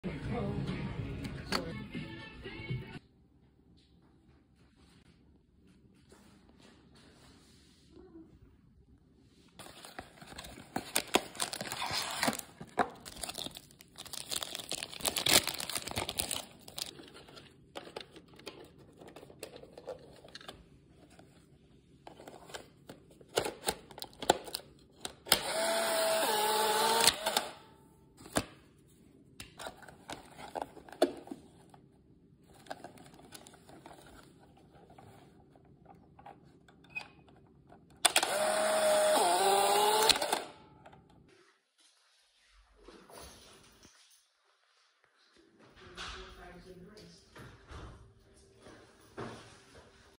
Reloading new film into a Polaroid Sun600 📷 ASMR
All thanks to my cousin Dana and husband Obed for extra Polaroid film :)